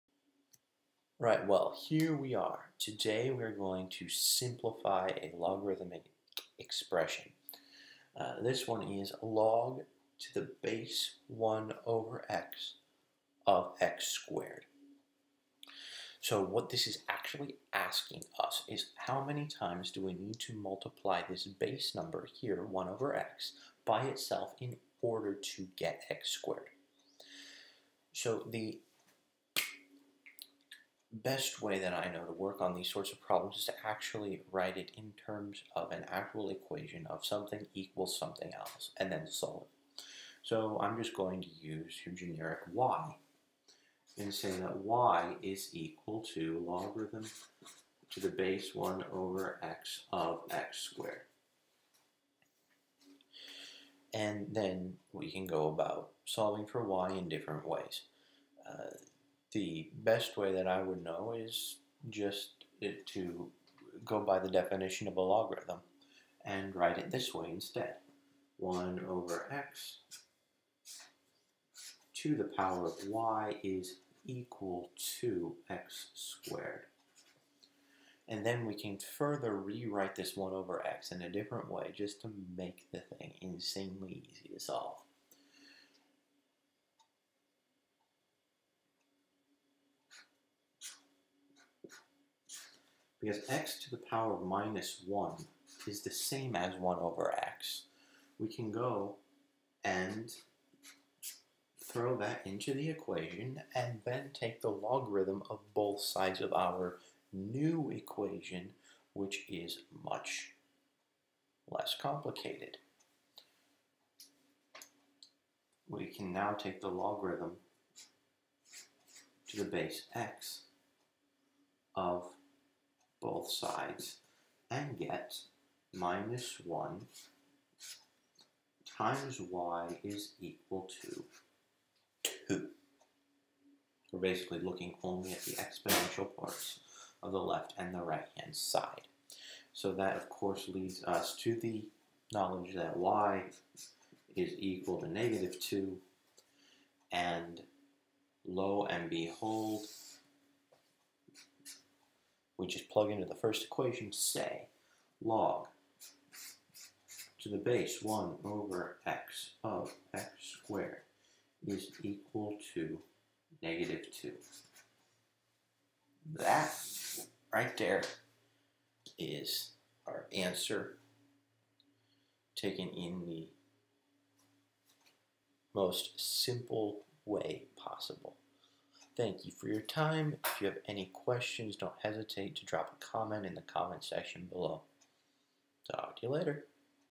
Simplifying a Logarithm with Variable Base
A little project for Amazon Mechanical Turk using the definition of a logarithm. :)